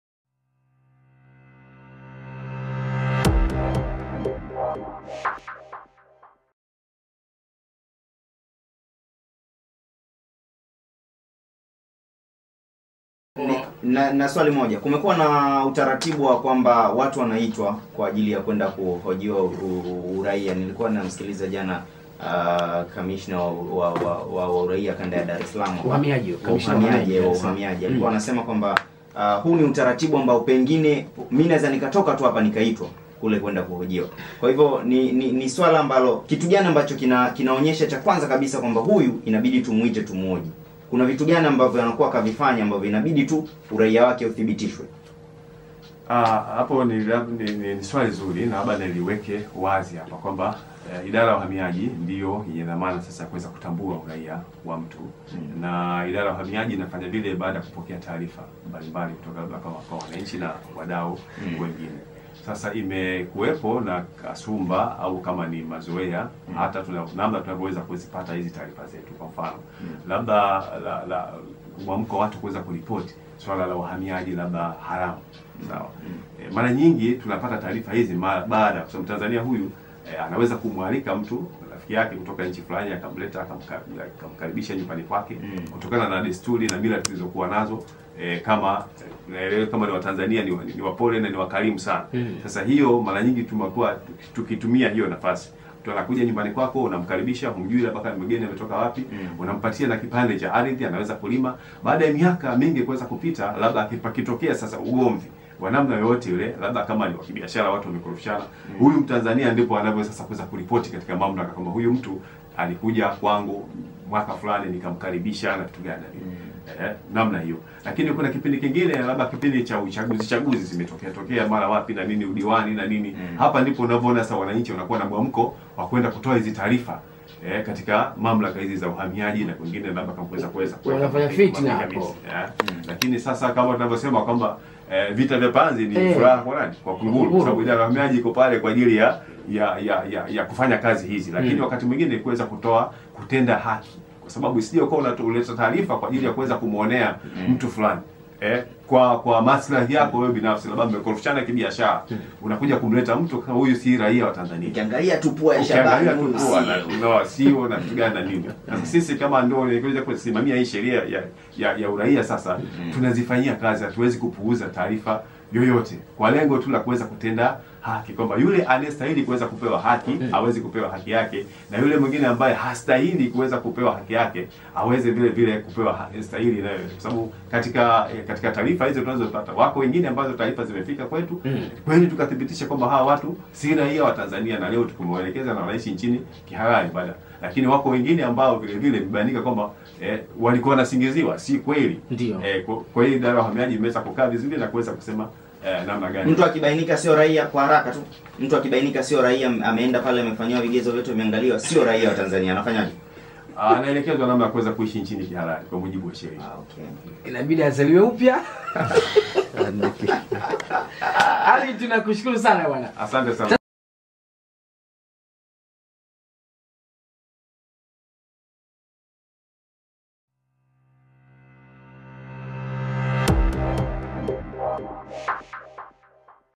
Majibu ya uhamiaji kuhusu watu kuhojiwa uraia wao..
msikilize hapa msemaji mkuu kutoka idara ya uhamiaji ndani ya eabreakfast...